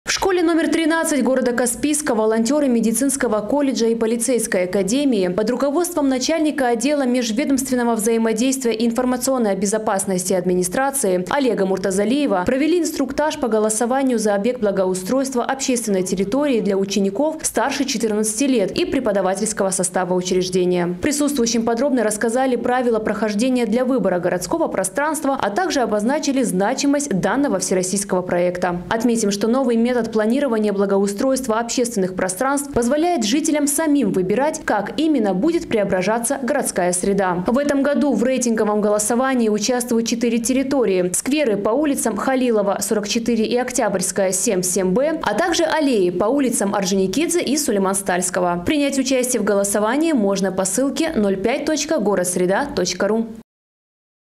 Волонтеры Каспийска провели инструктаж по голосованию за общественную территорию в школе № 13
В школе номер 13 Каспийска волонтеры медицинского колледжа и полицейской академии под руководством начальника отдела межведомственного взаимодействия и информационной безопасности Олега Муртузалиева провели инструктаж по голосованию за объект благоустройства общественной территории для учеников, старше 14 лет, и преподавательского состава учреждения. Присутствующим подробно рассказали правила прохождения для выбора городского пространства, а также обозначили значимость данного Всероссийского проекта.

В этом году в рейтинговом голосовании участвуют 4 территории: скверы по ул. М.Халилова, 44 и Октябрьская, 7-7 "Б, а также аллеи по ул. Орджоникидзе и С.Стальского.

Принять участие в голосование можно по ссылке 05.gorodsreda.ru